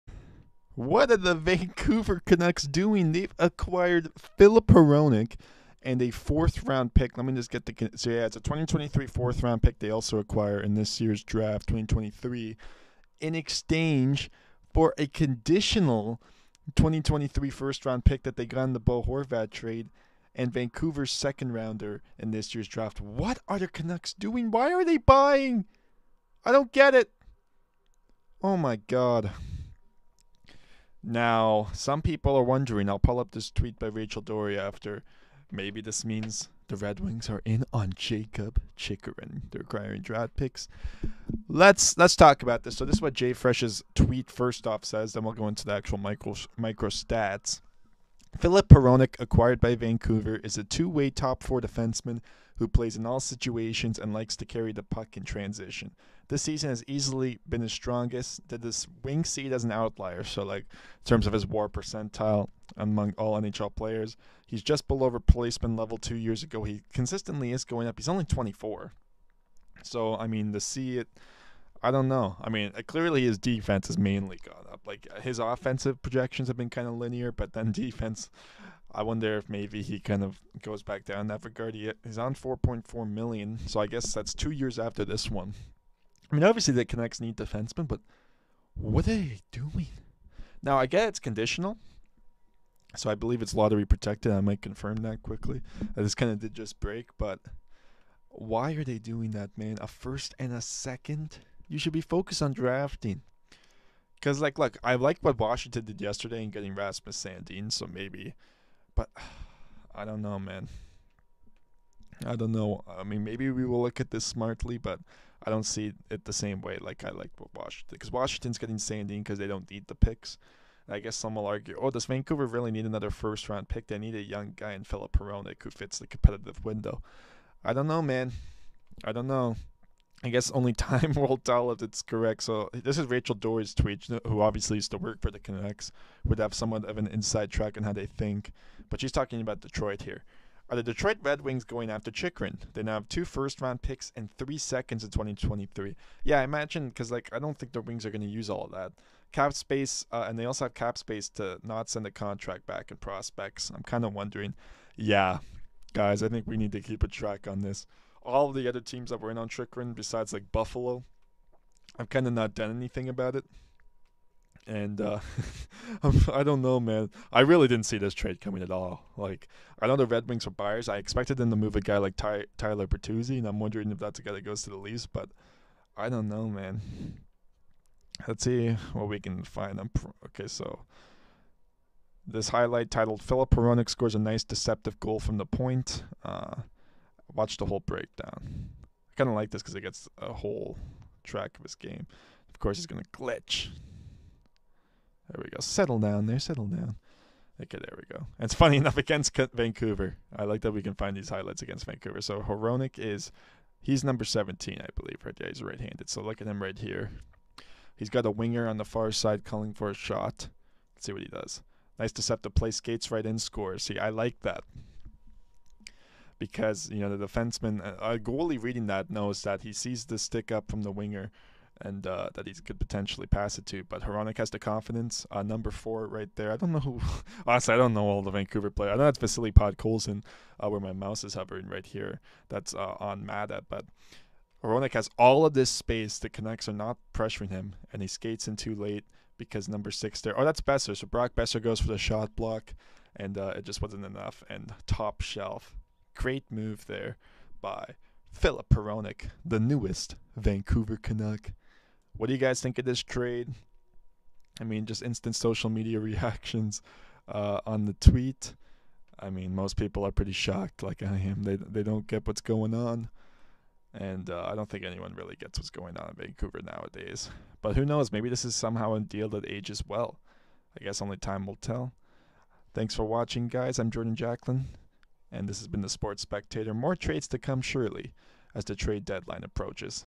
The Vancouver Canucks have no clue what they're doing...
The Vanouver Canucks acquired Filip Hronek, and we must all ask ourselves, WHY!!!